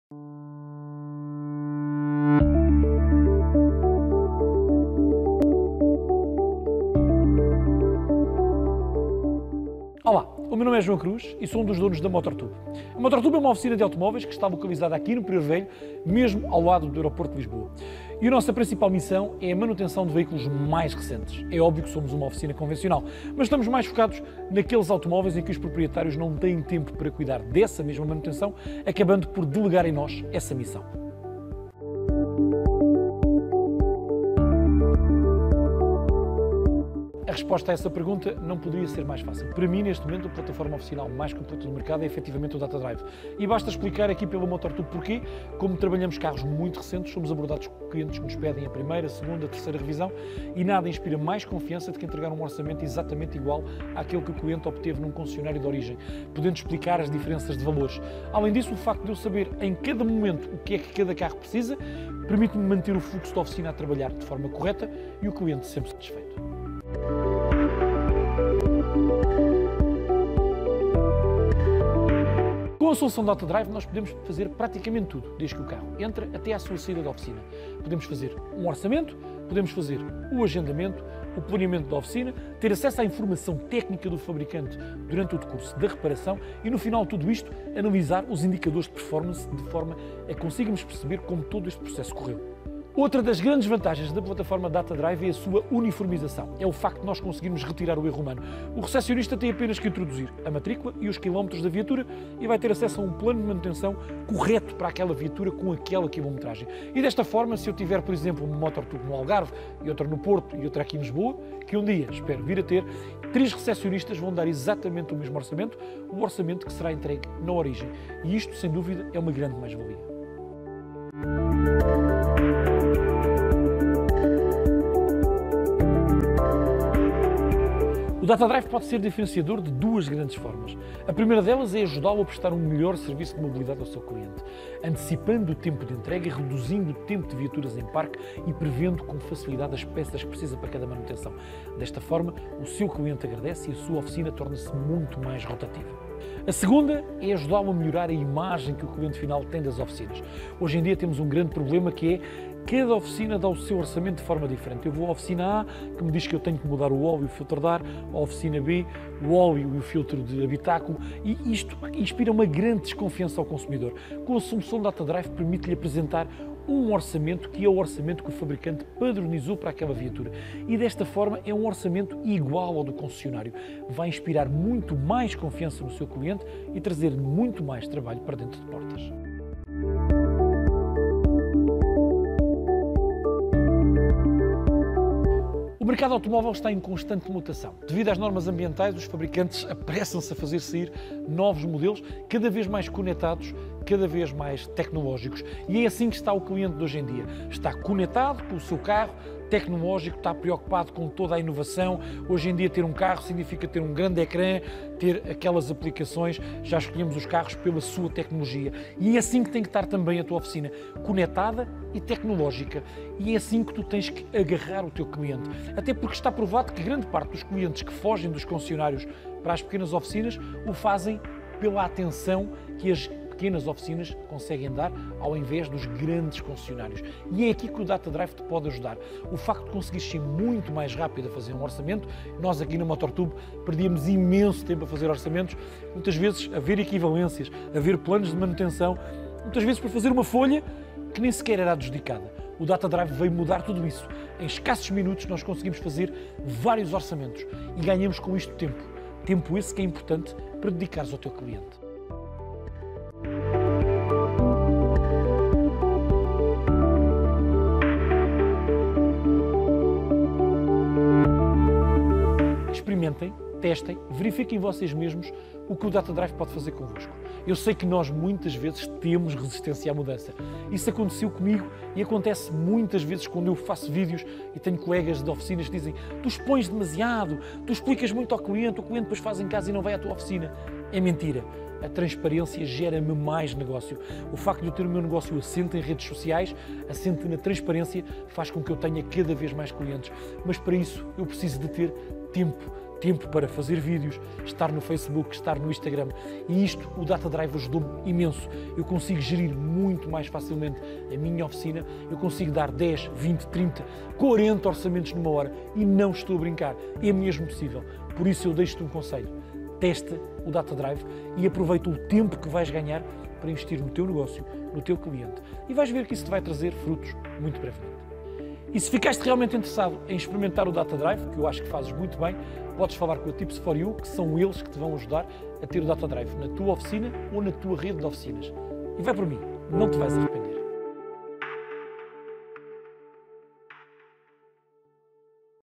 TipsTalk com João Cruz | Motortube
A Tips4y é uma empresa tecnológica que cria o melhor conhecimento sobre a identidade do automóvel para oferecer soluções inovadoras e transversais com impacto na mobilidade e experiência de uso de todo o ecossistema do setor automóvel.

00:00 Intro
00:06 Apresentação Motortube (João Cruz)
00:33 Porque utiliza a Plataforma Datadrive?
01:08 O que se pode fazer com a solução Datadrive?
02:02 De que forma é que o Datadrive pode ser diferenciador?
03:00 Que impacto esta solução poderá vir a ter no futuro do sector?
04:15 Que sugestões daria às Oficinas com base na experiência da Motortube?